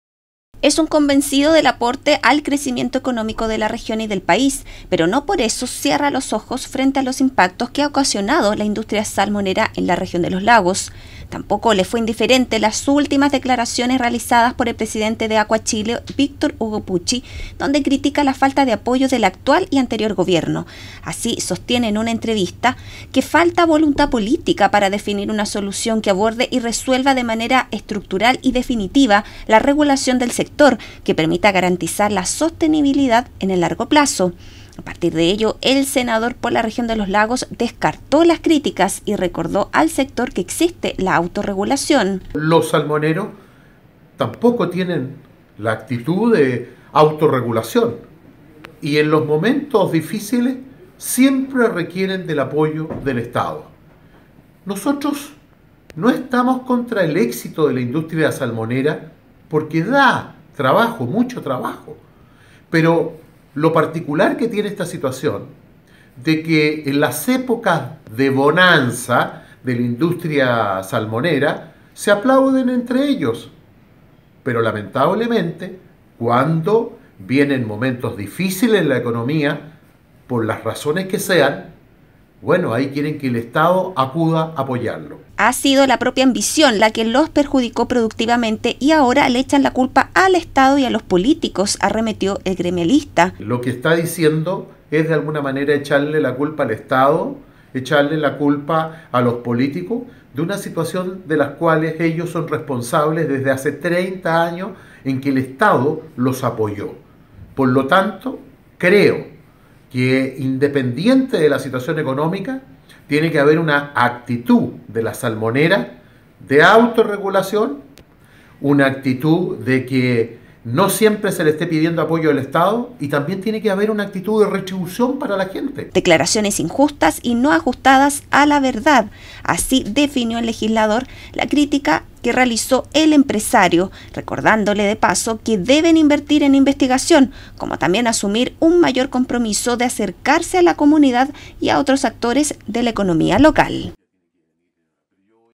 Senador Moreira critica a sector salmonero
El senador Iván Moreira descartó las críticas del presidente de AquaChile por una supuesta falta de apoyo del actual y anterior Gobierno a favor del sector salmonero. El legislador recordó que la industria ha carecido de autorregulación, lo que sumado a la ambición se auto perjudicaron productivamente.